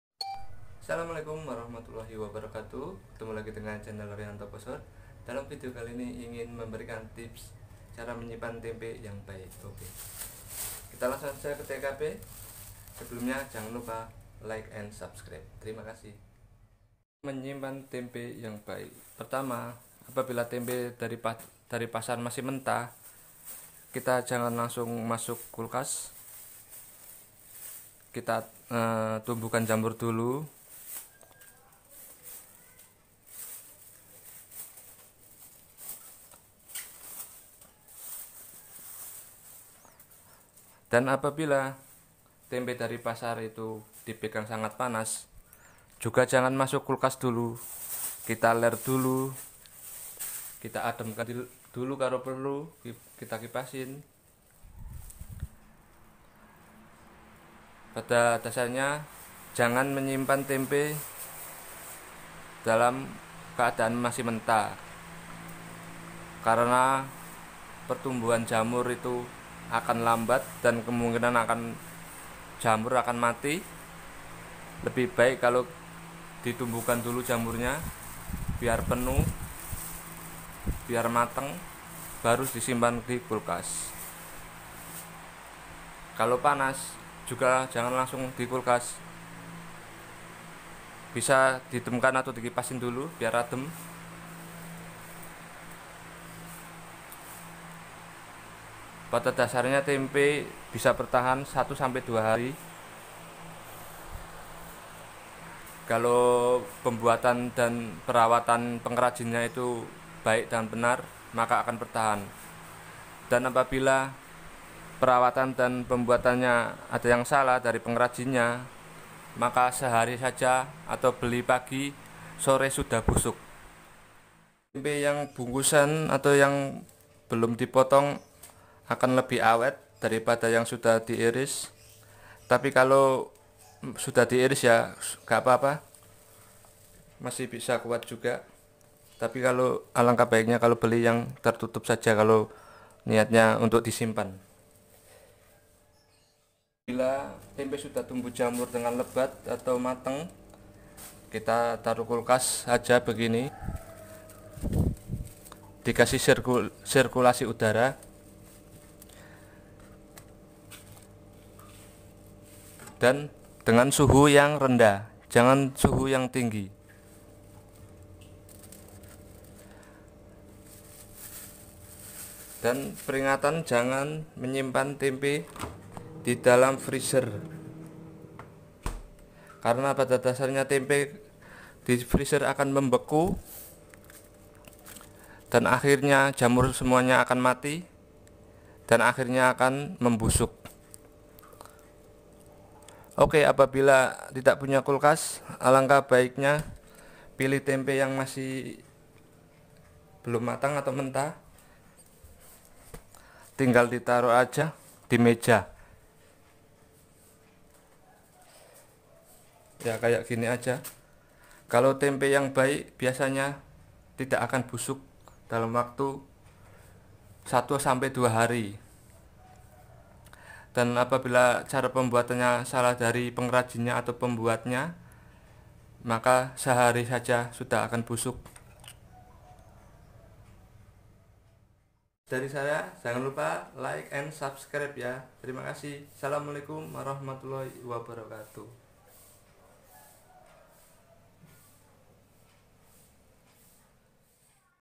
Tips menyimpan tempe yg baik
Tips menyimpan tempe yg baik,tips ini biasa saya lakukan sendiri dirumah.